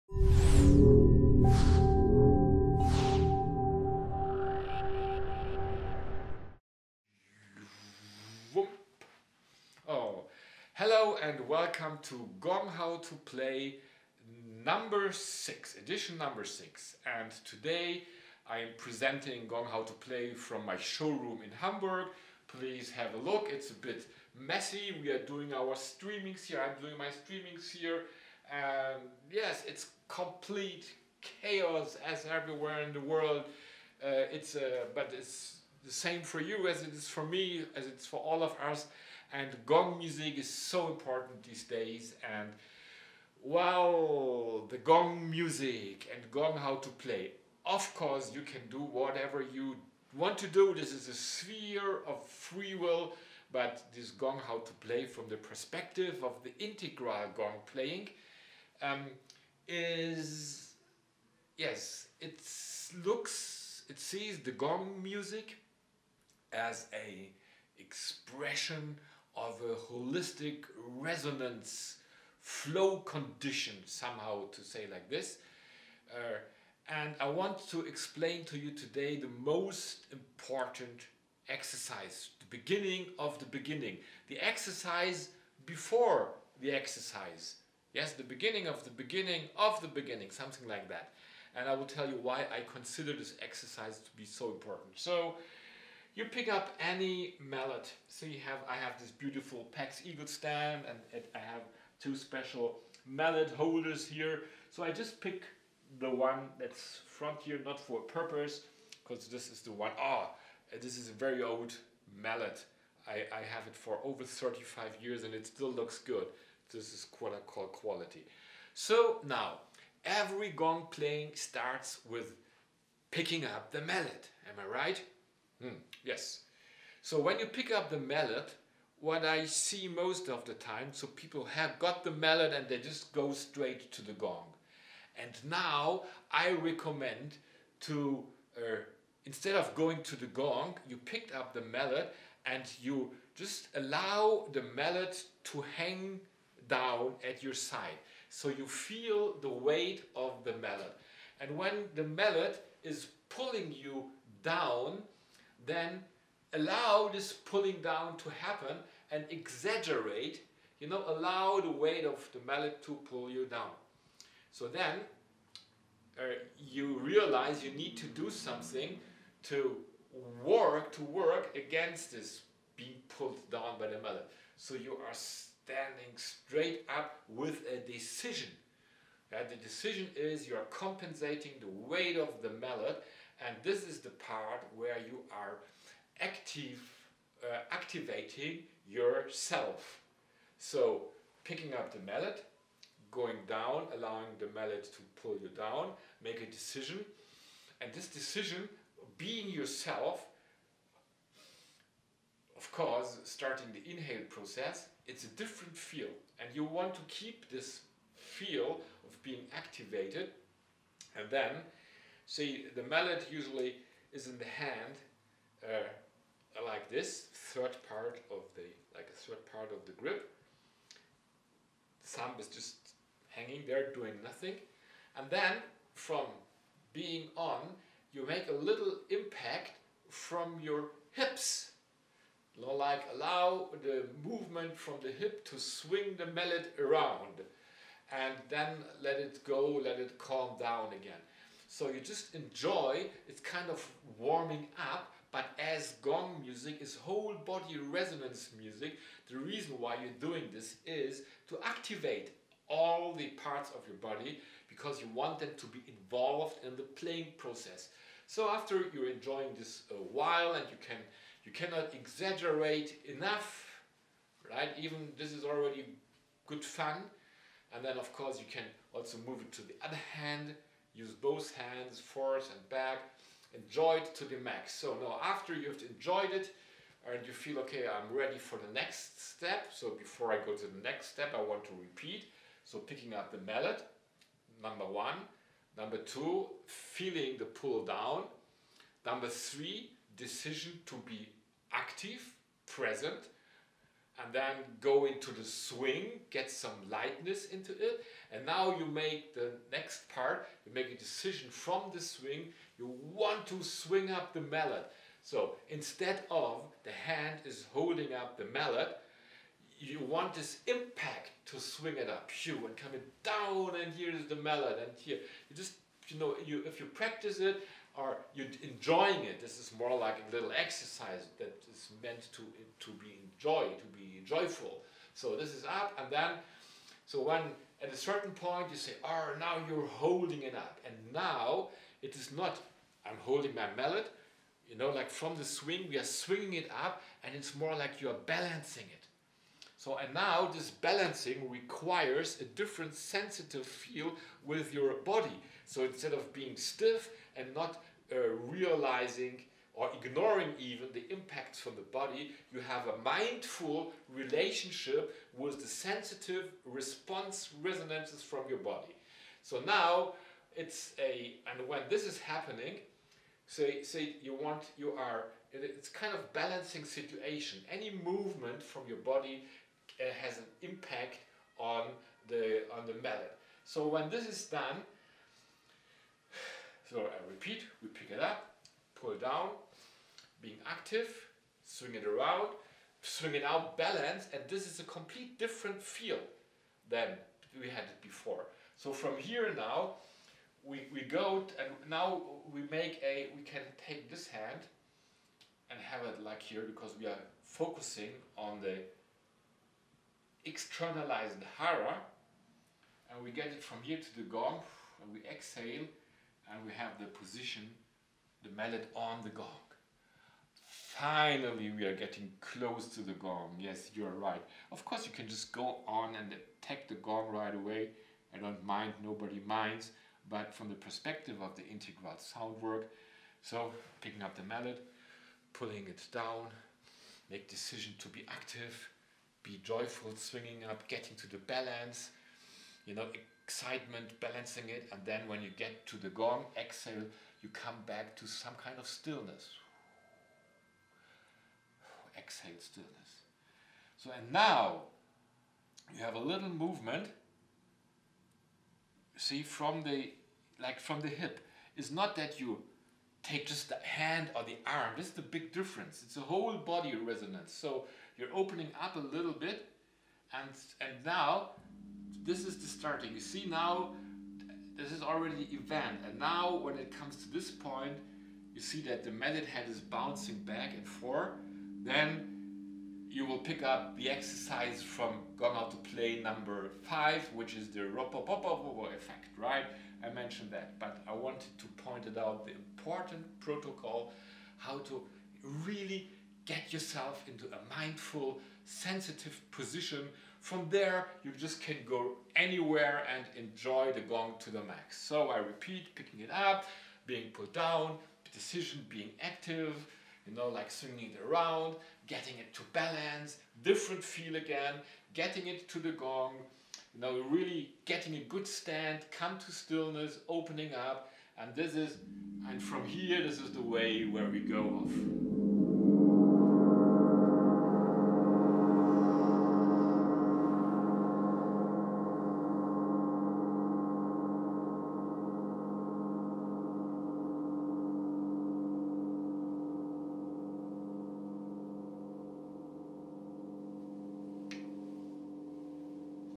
Gong how to play #6
This basic gong-playing exercise offers an organic access to the idea of authentic mindful gong playing. II will try to describe the moment of the beginning of the beginning of the beginning of any gong playing -  from the perspective of the integral sound work,
It's about activating the various levels of awareness, that can be, that could be a  part of any gong-playing; rather then playing from a mind space, or the average unaware space of a dumb dead frozen body, moving into the resonating vibrating space of a happy, healthy, sensitive body, which will make your personal gong playing much more fun and allow happiness to be a part of your playing and presentation feel.

This basic protocol of the integral gong playing is a lesson
 about having some great exciting fun with your body while swinging the mallet around. It is about transforming the concept of gravity pull into gravity swing.

I am looking so much forward to meet you again in the real life and play some great joyful soundmusic together with you.

But till, we keep on practising, and learning and upskilling.

Carpe Diem.